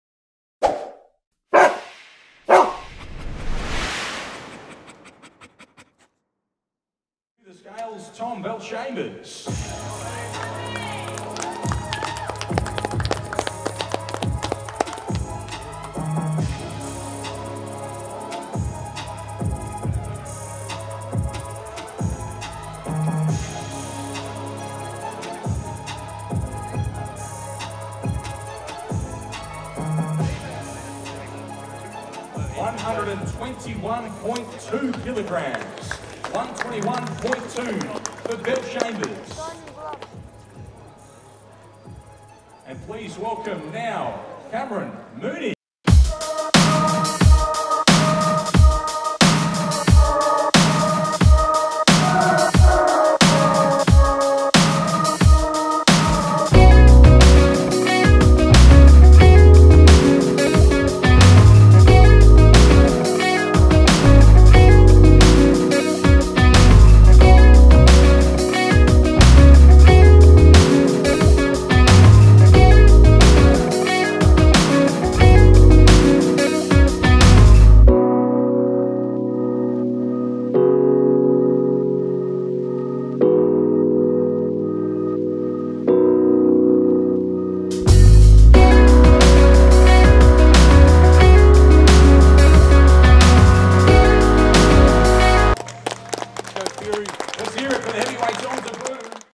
CAMERON MOONEY VS TOM BELLCHAMBERS - TENSE WEIGH- IN AND STARE DOWN!
Three-time AFL premiership winner Cameron Mooney and former Essendon star Tom Bellchambers Weigh-In ahead of their fight set to take place on the undercard of Joseph Parker vs Faiga Opelu, May 24 in Melbourne.